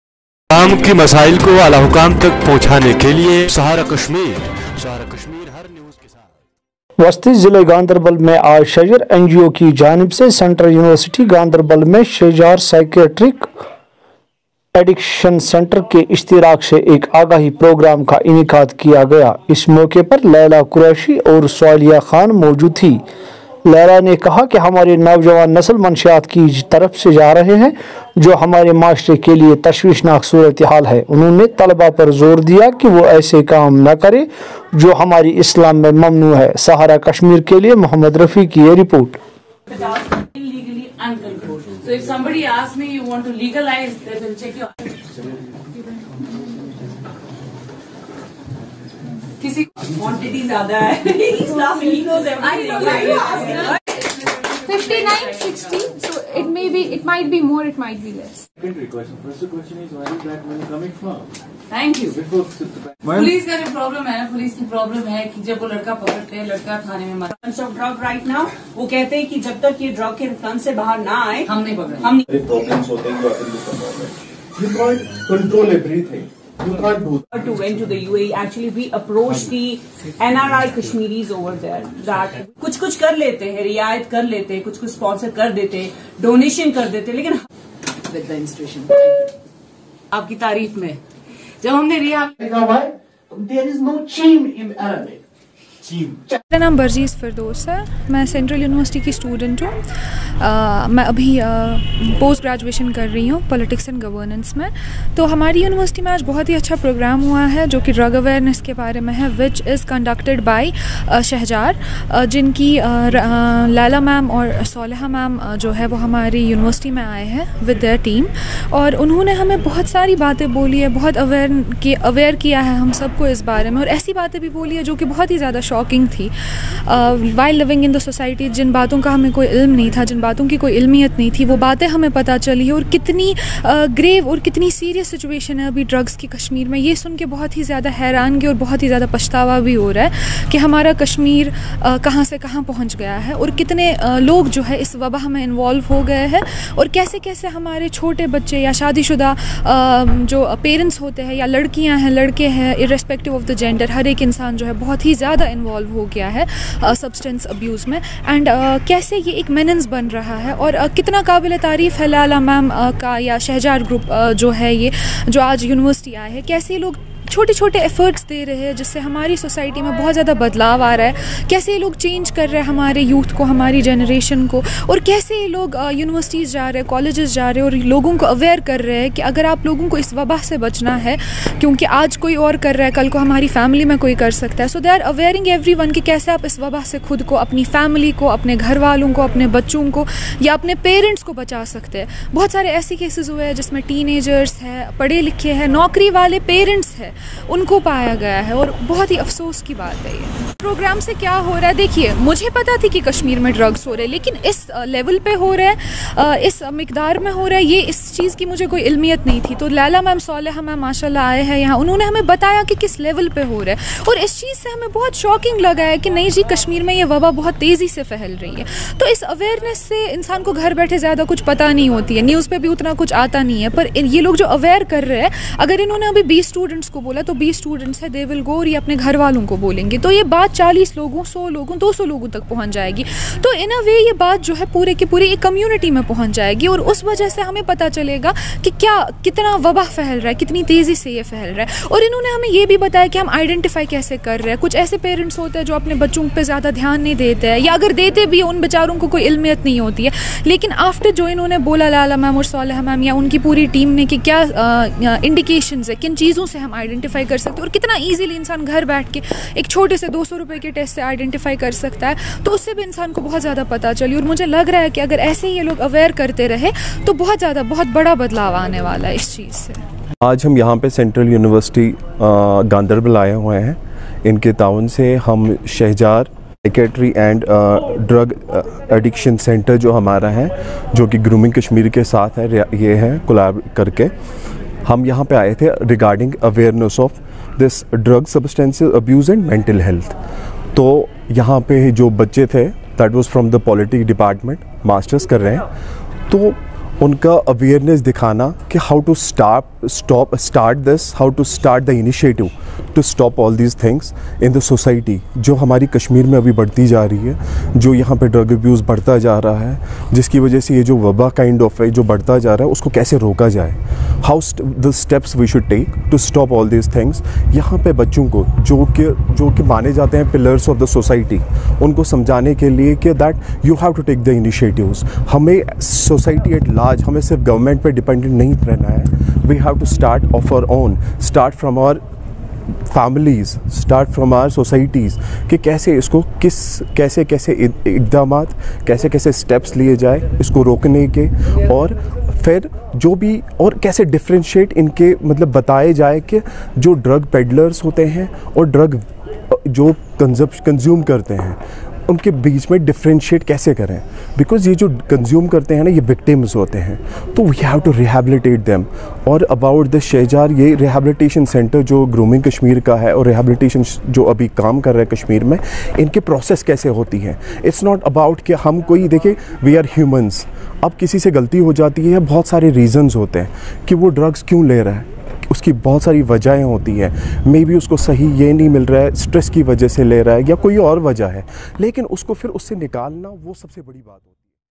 Awareness programme was organised by Shehjar (NGO)
Awareness programme was organised by Shehjar (NGO) with collaboration of Shehjar Psychiatric and de addiction centre at Central University  Ganderbal
Report By Dar Mohd Rafi